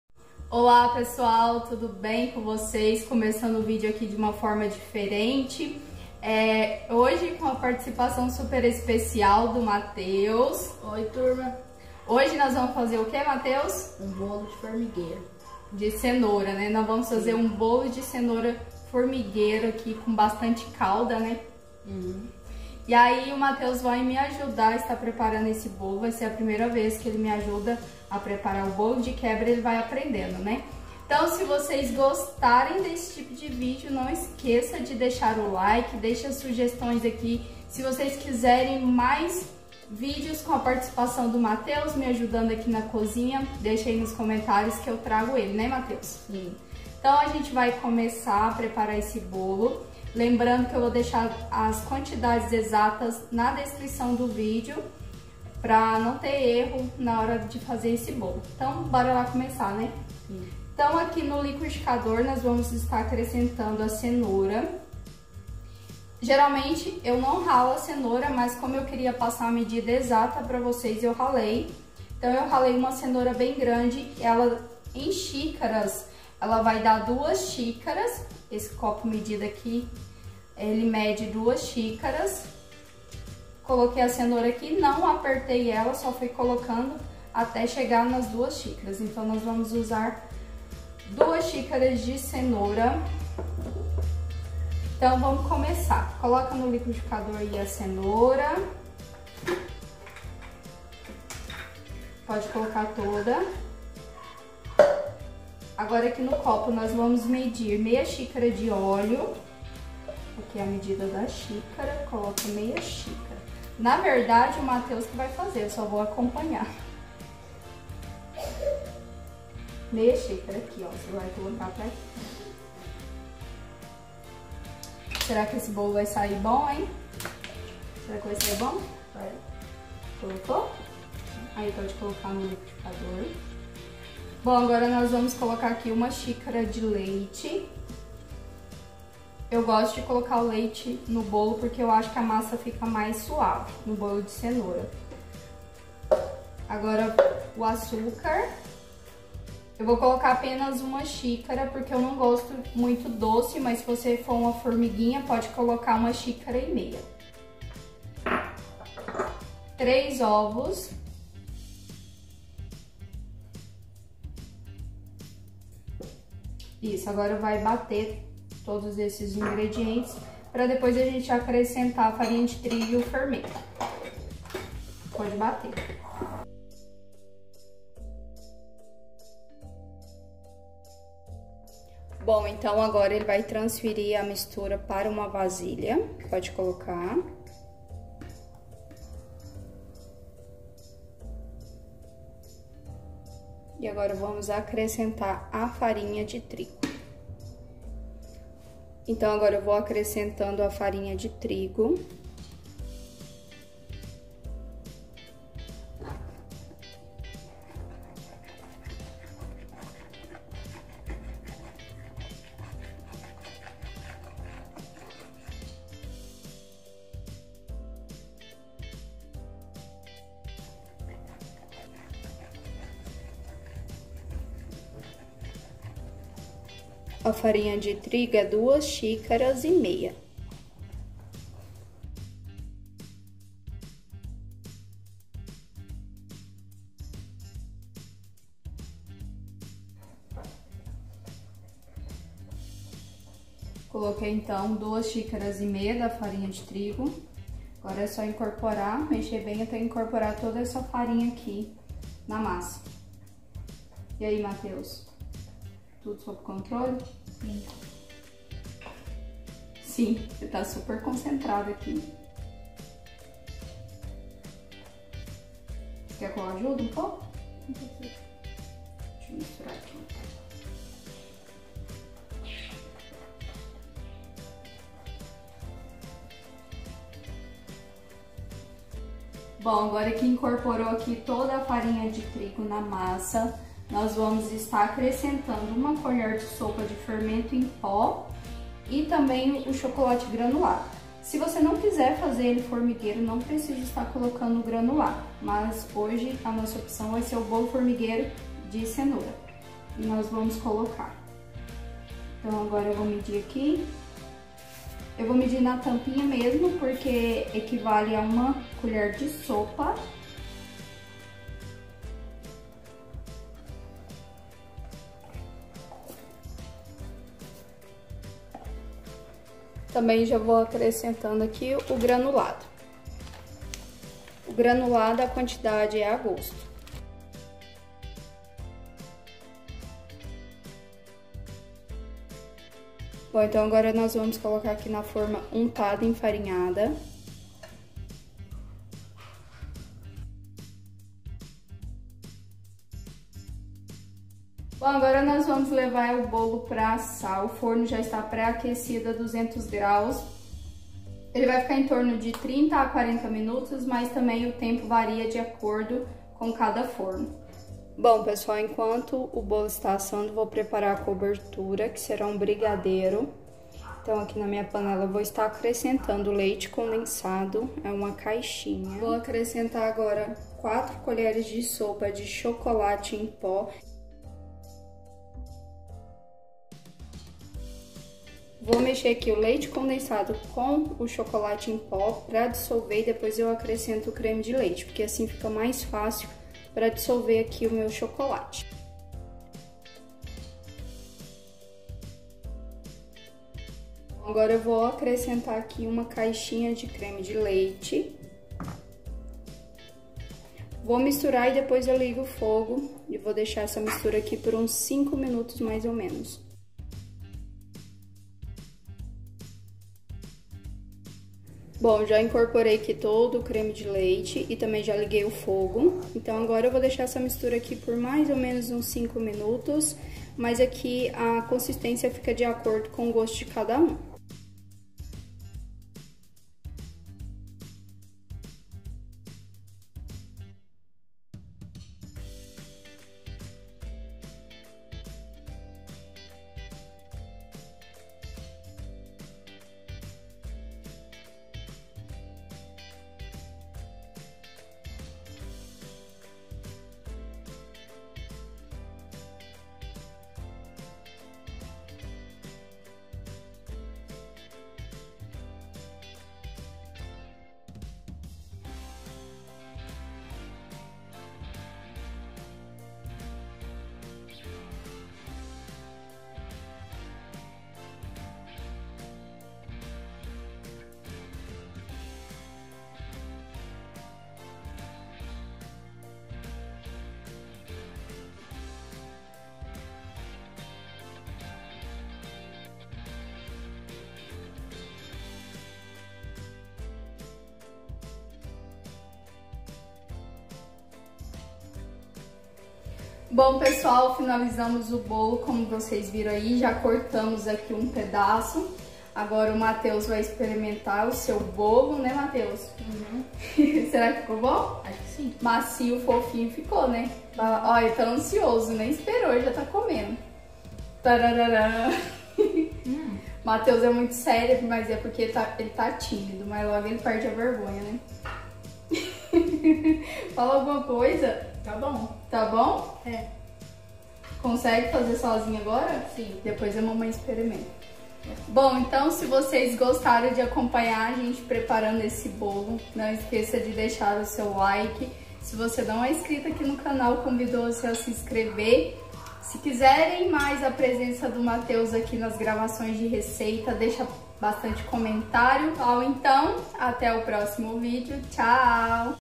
VOCÊ JA FEZ BOLO DE CENOURA ASSIM? O MAIS FOFINHO! FÁCIL E RÁPIDO
Ingredientes
2 xícaras de cenoura ralada
2 e 1/2 Xícaras de Farinha de trigo
1 xícara de Leite
1/2 Xícara de óleo
1 colher de sopa de fermento em pó
3 ovos

Chocolate Granulado a gosto

Cobertura
1 caixa de leite condensado
1 caixa de creme de leite
4 colheres de chocolate em pó